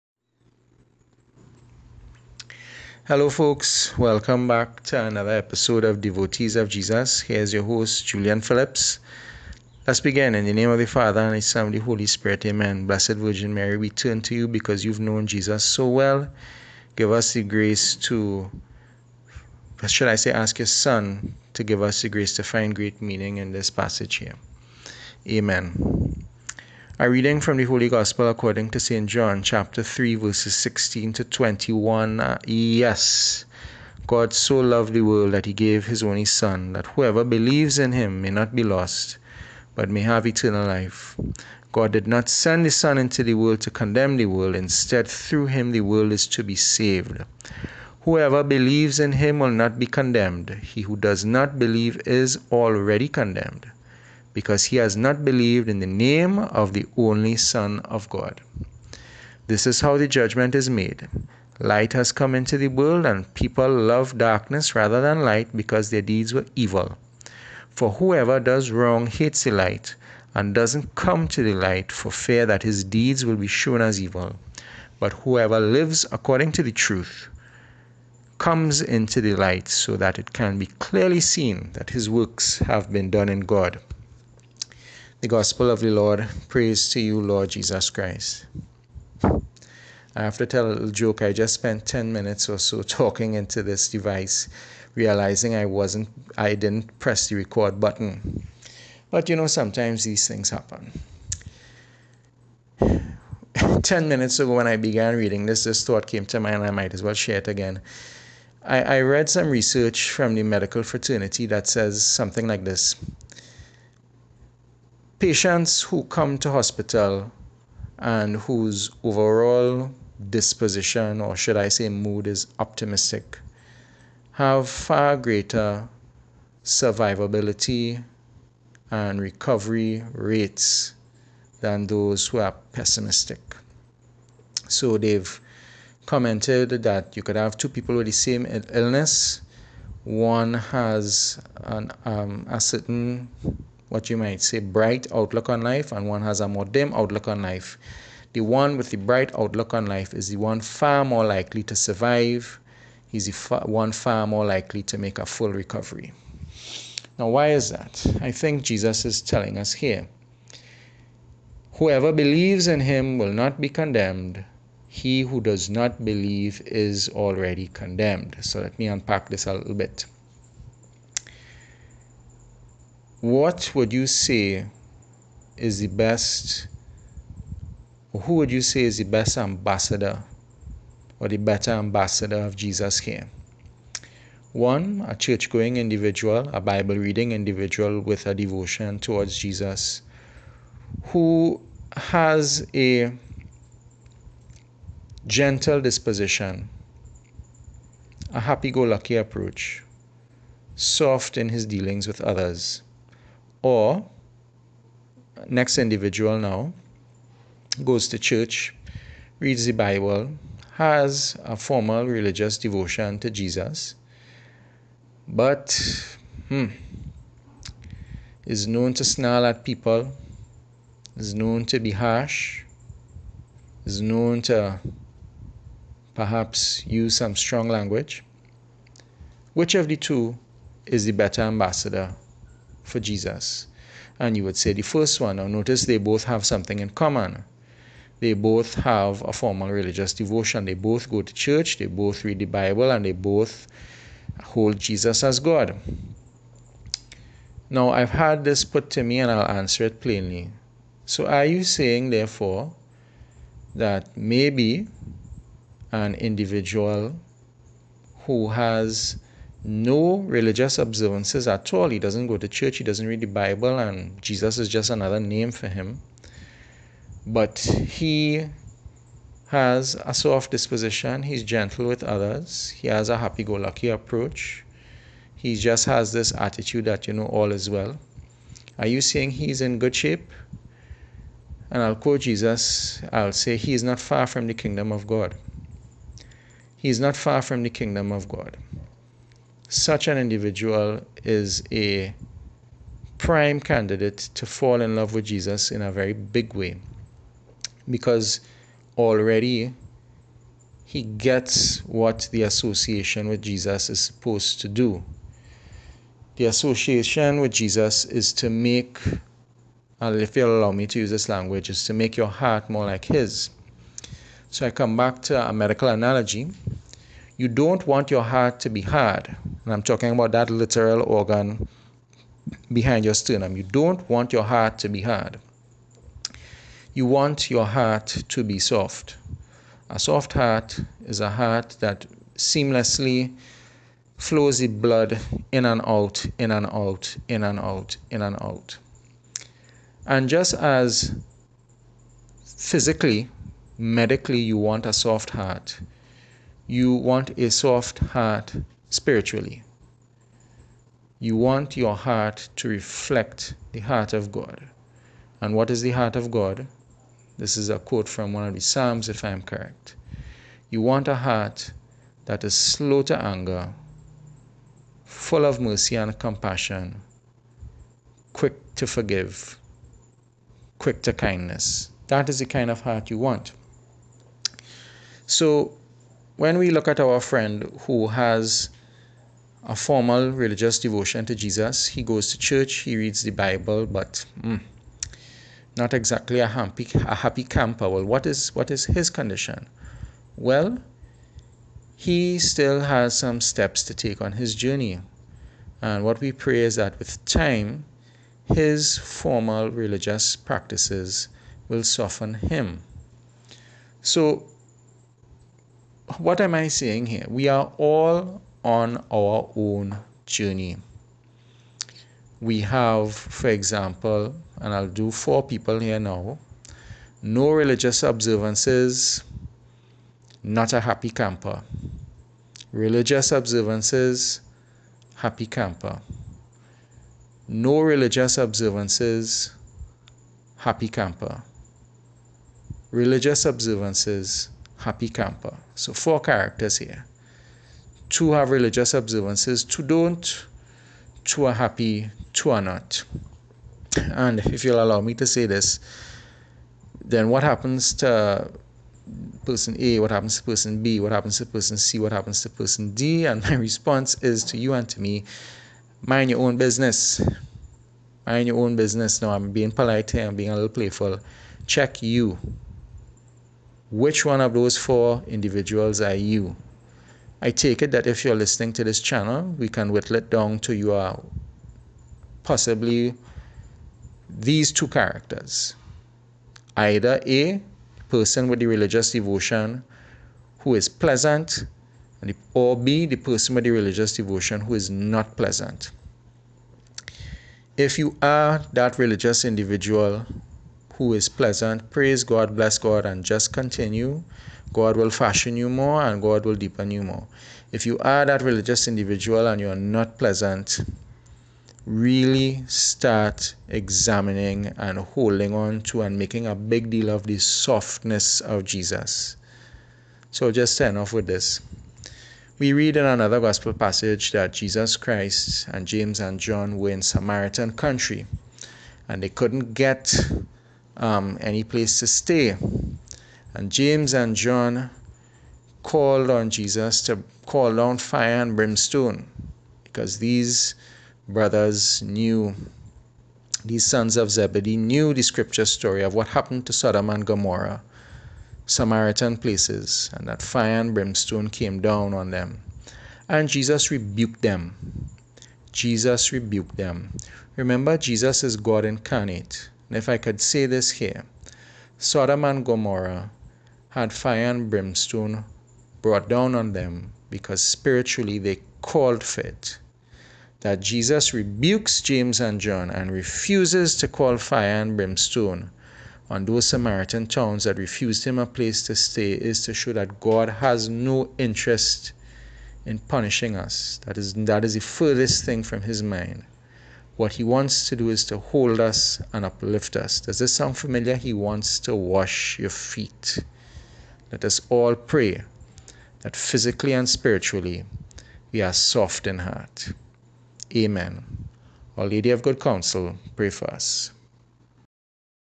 Episode 65 Devotees of Jesus
Using St. John, Chapter 3, Verses 16 to 21, we ask what is your disposition.